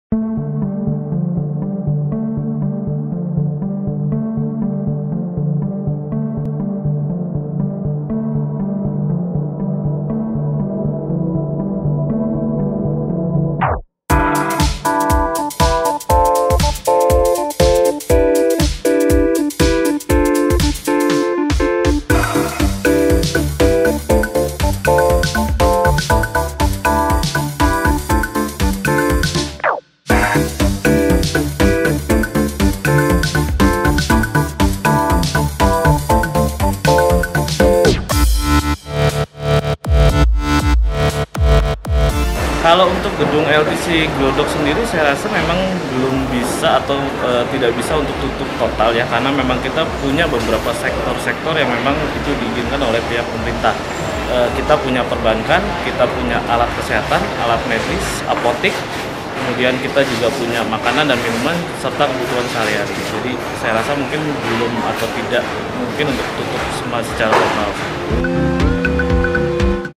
Memasuki PSBB Total, LTC Glodok Perketat Protokol Kesehatan
LTC Glodok sudah siap dengan kebijakan PSBB total Jakarta yang akan dijalankan 14 Agustus 2020. Sejumlah fasilitas telah diubah dan disesuaikan
dengan protokol kesehatan yang ada. Seperti misalnya, pengecekan suhu yang tidak lagi menggunakan thermogun, melainkan sensor monitor.
Lift pun juga dimodifikasi agar pengunjung tak perlu menyentuh tombol lantai. Pengguna lift hanya perlu menyebutkan
lantai tujuannya dengan perintah suara. Selain itu, petugas akan terus berkeliling mengingatkan pengunjung dan pegawai untuk selalu memakai masker dan tetap menjaga jarak aman.

DATA: Berbagai Sumber | Video: Miftahul Munir |
NASKAH: Fandi | VIDEO EDITOR: Aziez Maulana | SEPTEMBER 2020